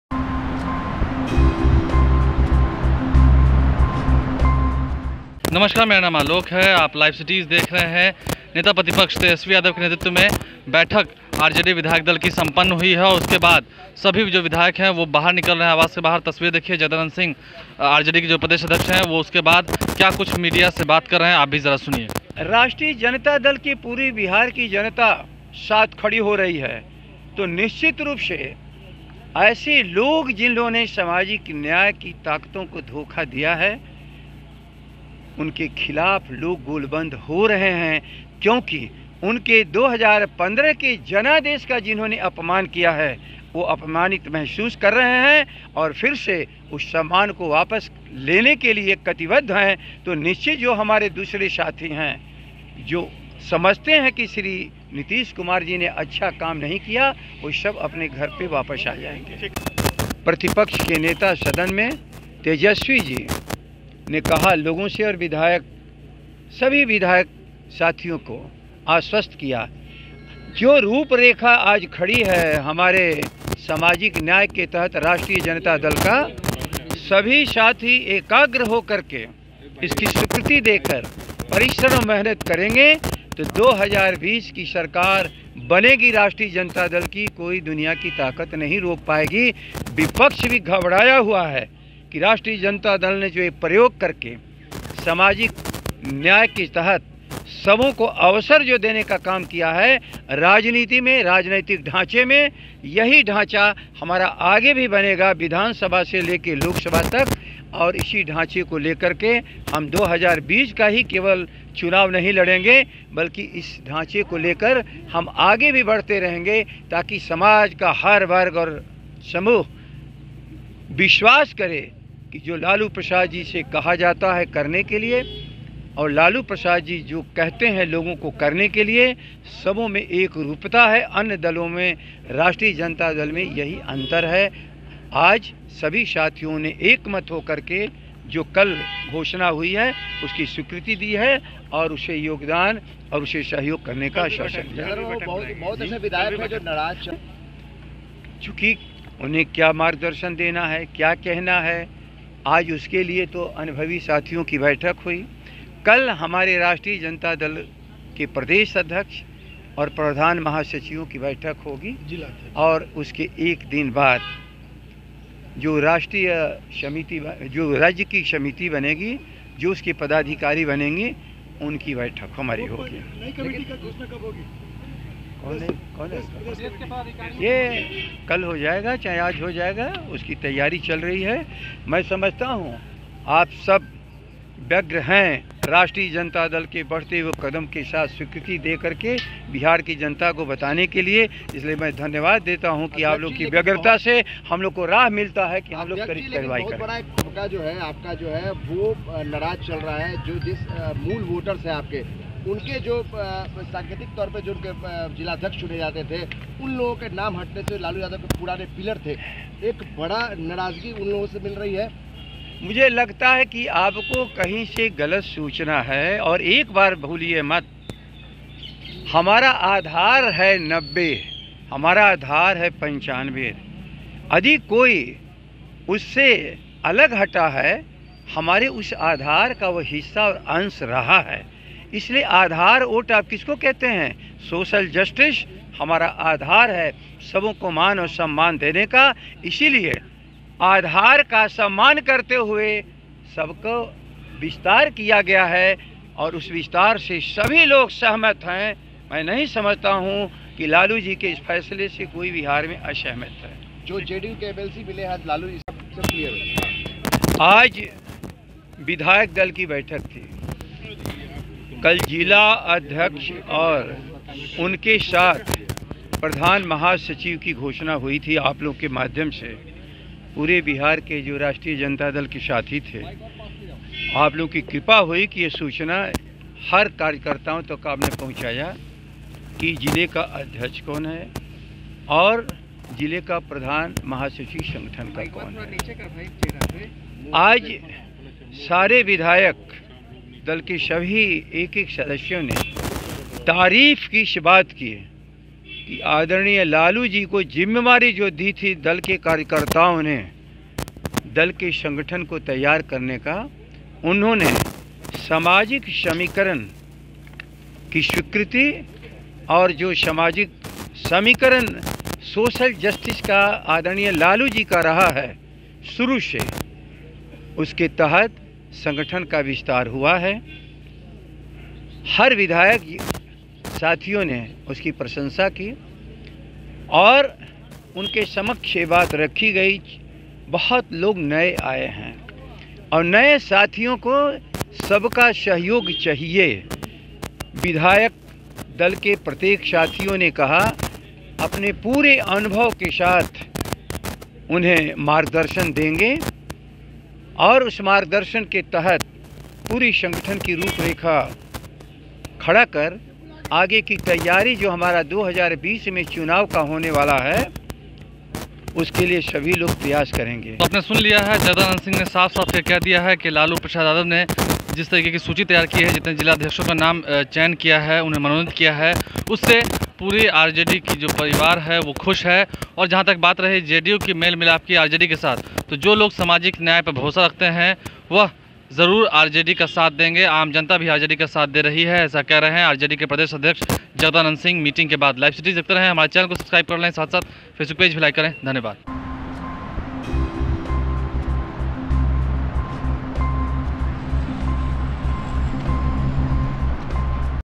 Tejashwi Yadav के साथ बैठक के बाद बोले Jagdanand Singh, Lalu Yadav की RJD के साथ पूरा Bihar खड़ा है
Tejashwi Yadav के साथ बैठक के बाद बोले Jagdanand Singh, Lalu Yadav की RJD के साथ पूरा Bihar खड़ा है | LiveCities